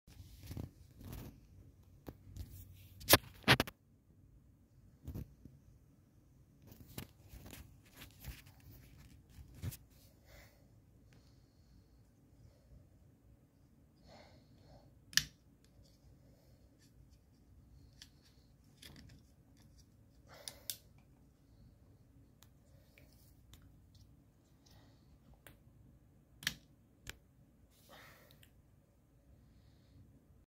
i struk the craziest deal on Amazon.
this mattel T-Rex fossil stem kit is a dig up you dig it out amd build it. it was from 2018 and is on amazon for.
$10.00
 yep. its worth alot on ebay but not amazon.